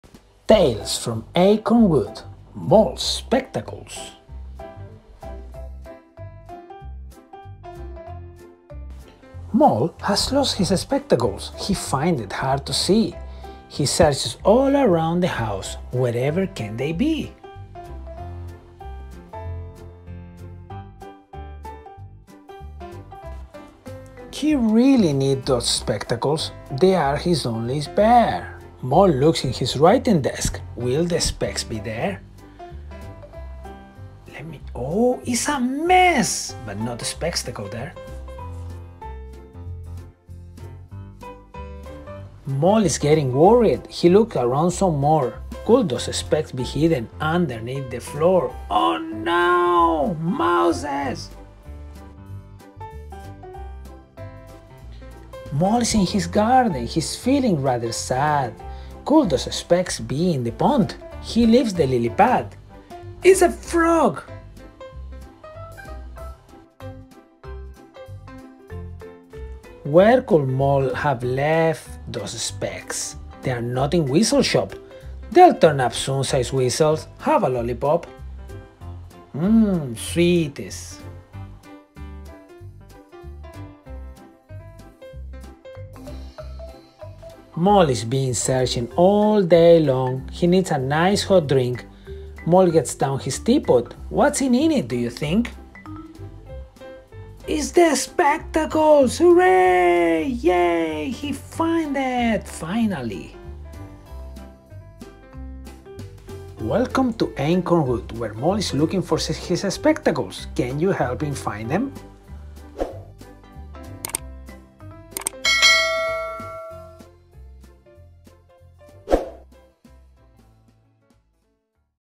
“Mole’s spectacles” - Tales from Acorn Wood - Reading books aloud for kids
This time I’m bringing for all of you “Mole’s spectacles” from the series “Tales from Acorn Wood”

Check other books from our channel ☺️